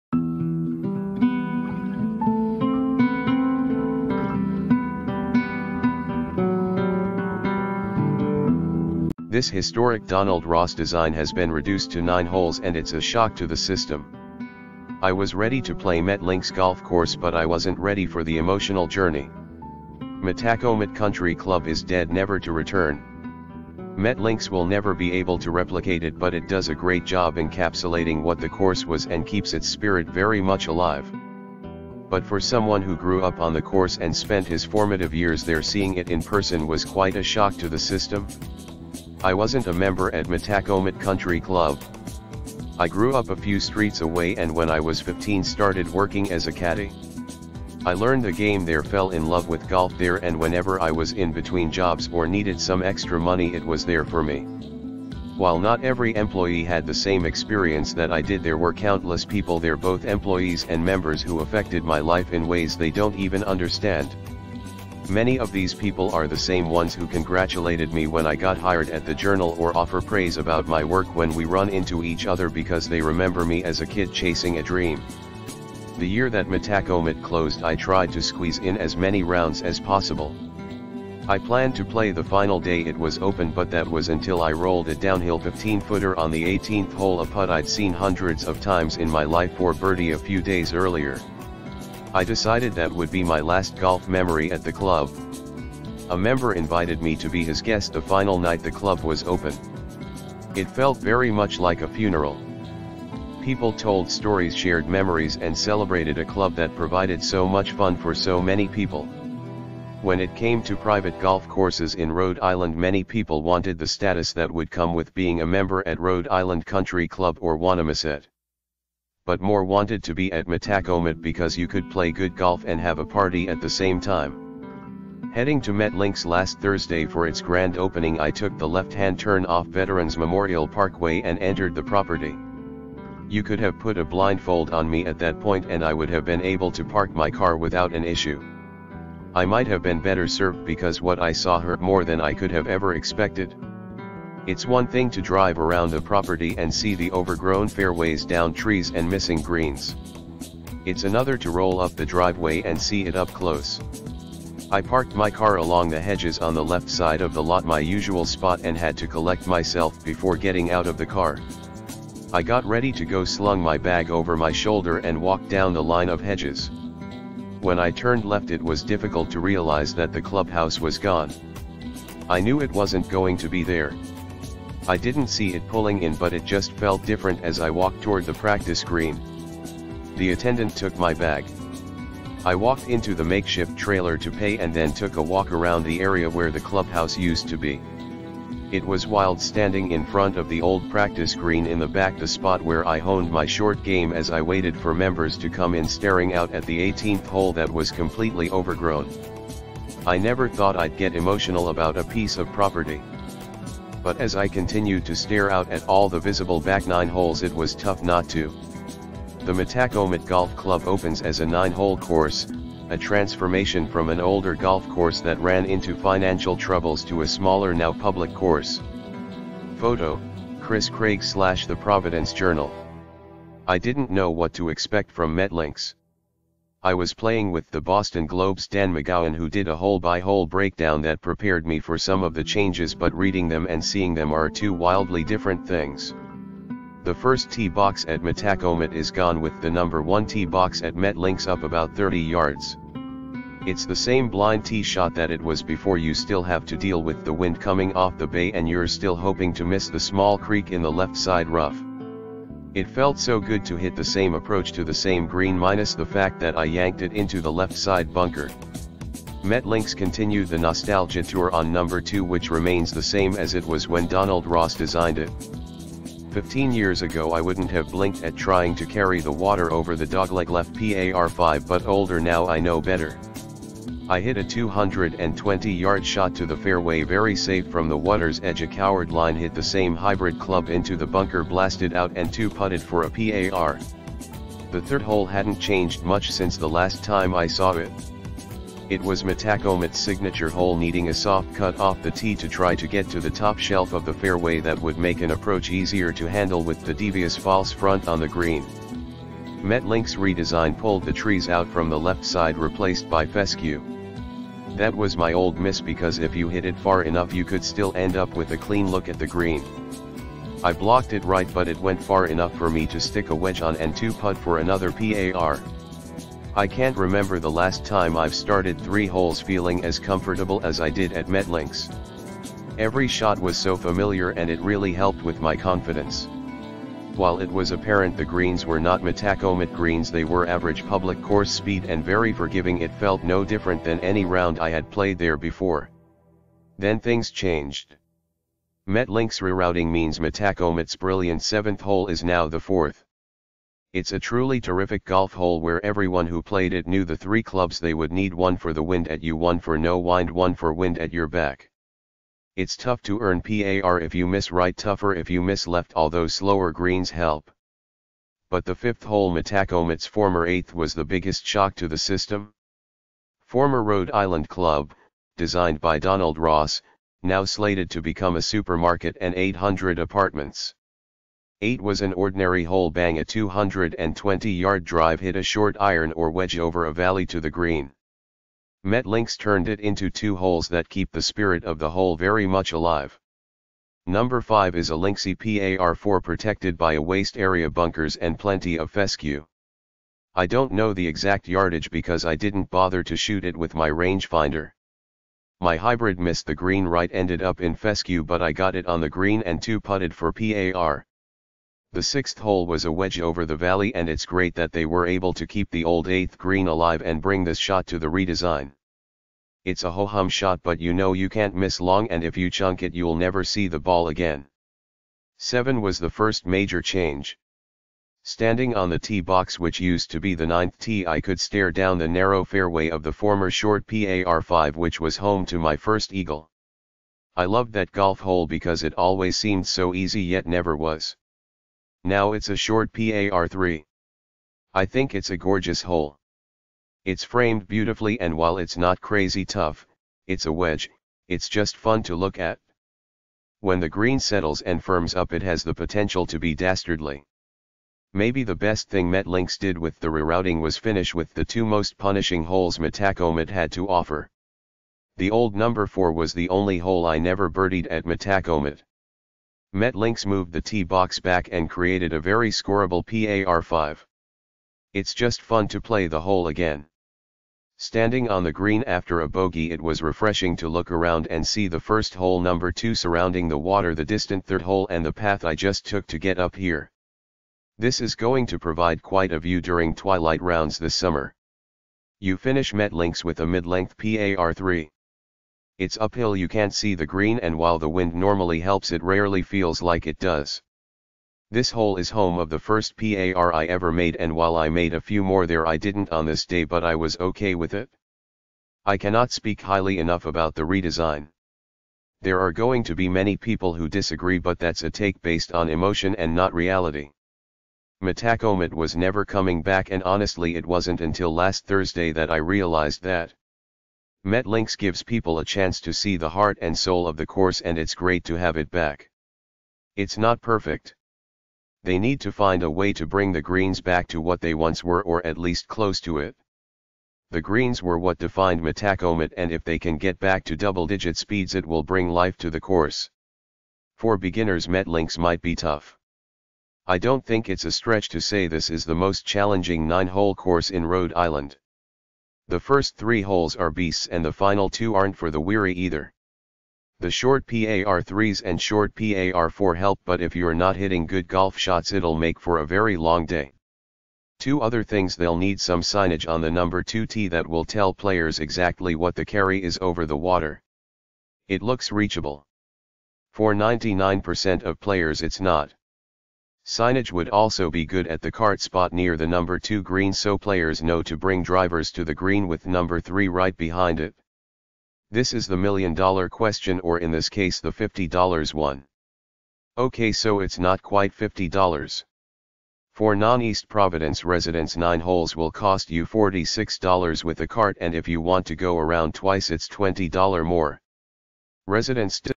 🔴This historic Donald Ross design has been reduced to nine holes and it’s a ‘shock to the system’👀
The first tee box at Metacomet is gone, with the No. 1 tee box at MetLinks up about 30 yards. It’s the same blind tee shot that it was before — you still have to deal with the wind coming off the bay and you’re still hoping to miss the small creek in the left-side rough. It felt so good to hit the same approach to the same green, minus the fact that I yanked it into the left-side bunker.

MetLinks continued the nostalgia tour on No. 2, which remains the same as it was when Donald Ross designed it. Fifteen years ago, I wouldn’t have blinked at trying to carry the water over the dogleg-left par 5, but older now, I know better. I hit a 220-yard shot to the fairway, very safe from the water’s edge, a coward line, hit the same hybrid club into the bunker, blasted out and two-putted for a par.